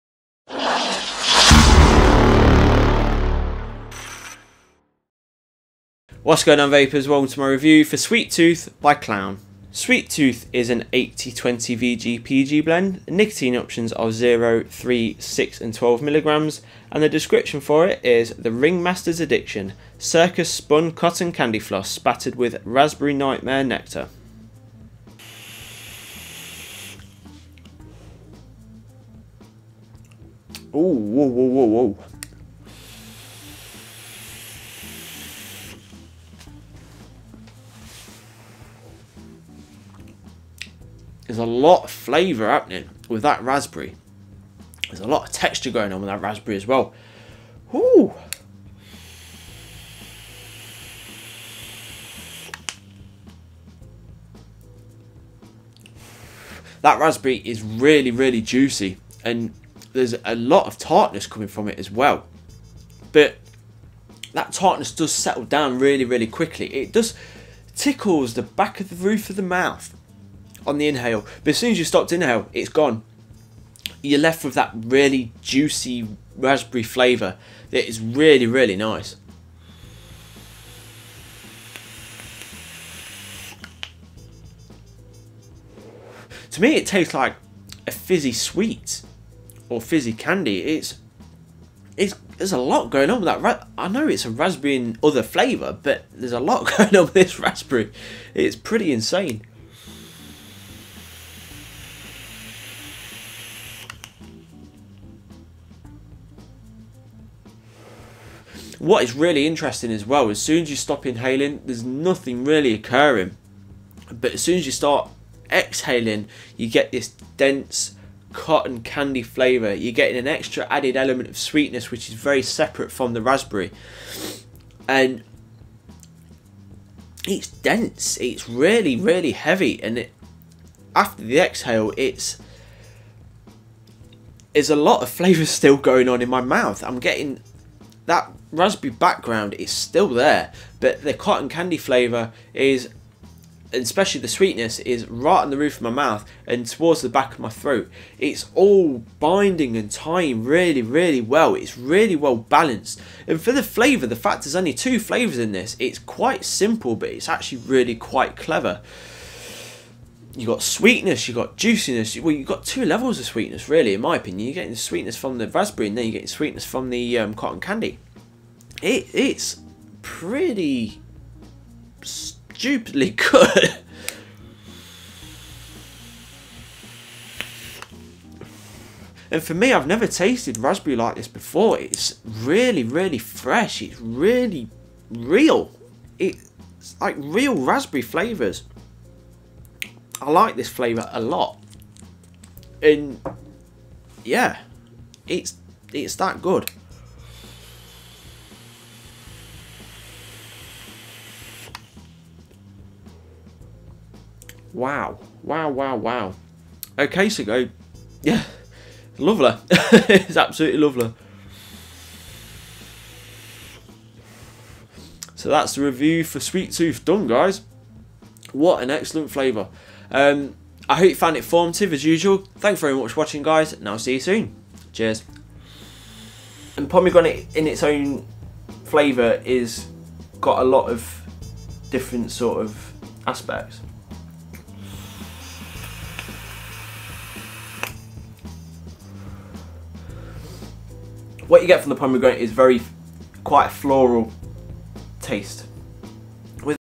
SWEET TOOTH e liquid REVIEW by Clown
A Kick ass label design and amazing flavour descriptions but does SWEET TOOTH taste as good as it looks?
Here is my e-liquid Review for Sweet Tooth by Clown